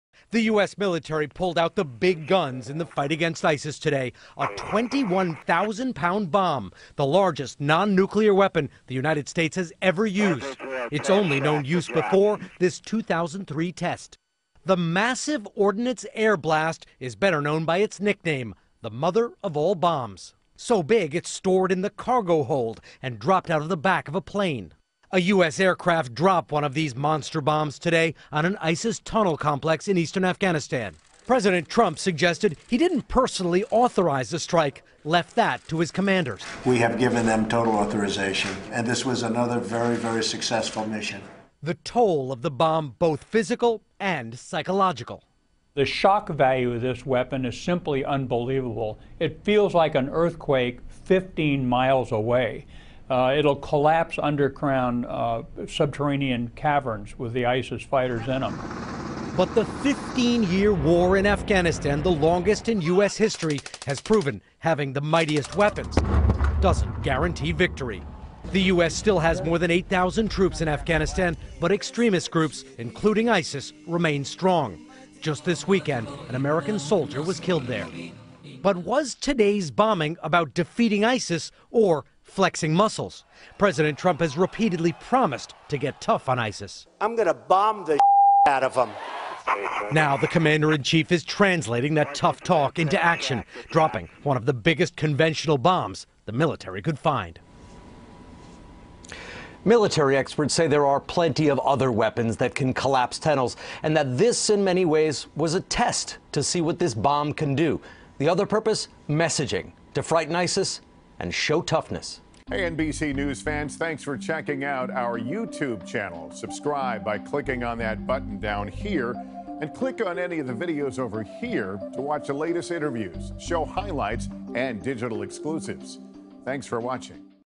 USA deja caer su mayor bomba no nuclear en combate
El ejercito de los estados unidos ha dejado caer su mayor bomba No Nuclear